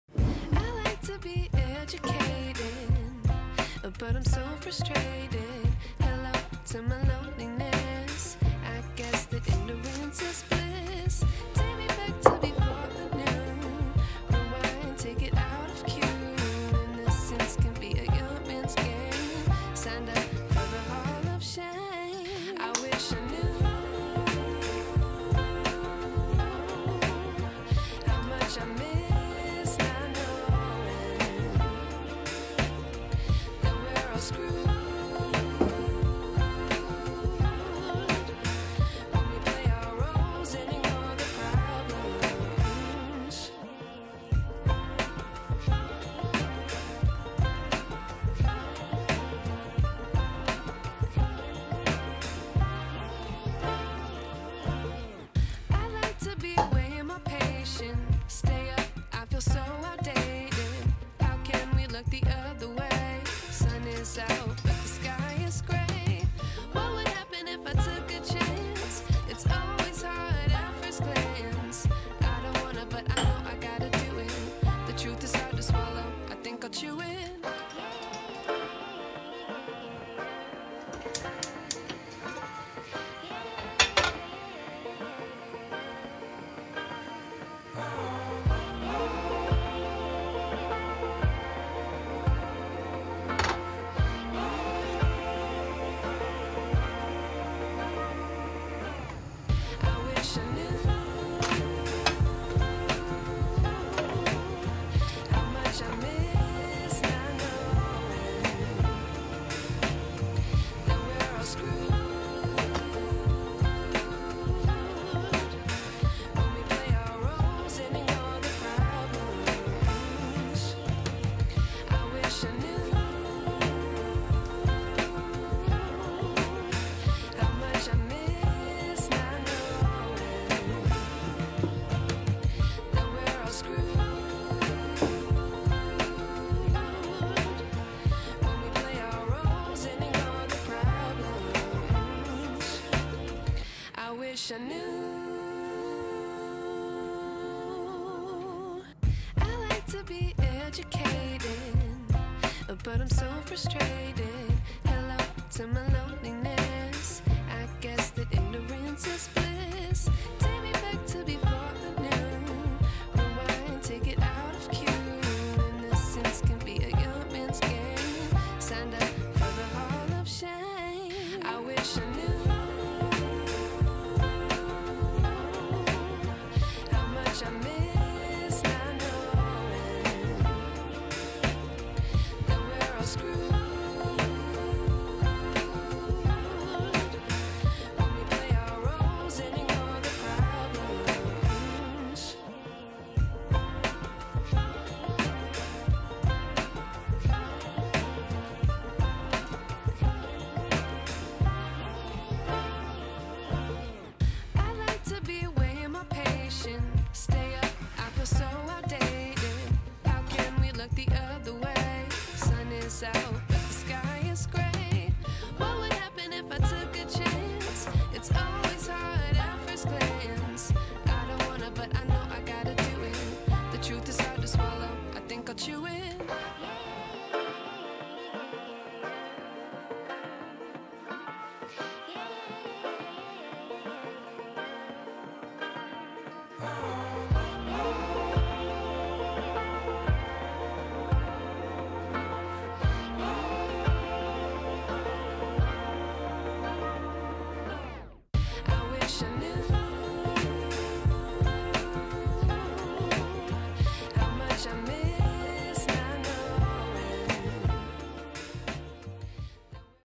БЛИННЫЕ РОЛЛЫ с бананами и НУТЕЛЛОЙ
Если ищите вкусный рецепт на Масленицу или хотите сделать блинный торт, то обратите внимание на этот рецепт блинов. Тут у нас блины на молоке, бананы и нутелла. И это не просто блины с бананами, а роллы из блинов! Очень вкусно!

Что нужно:

мука примерно 170-180 г
яйцо 1 шт
соль
сахар 1 ст л
разрыхлитель 1 ч л с горкой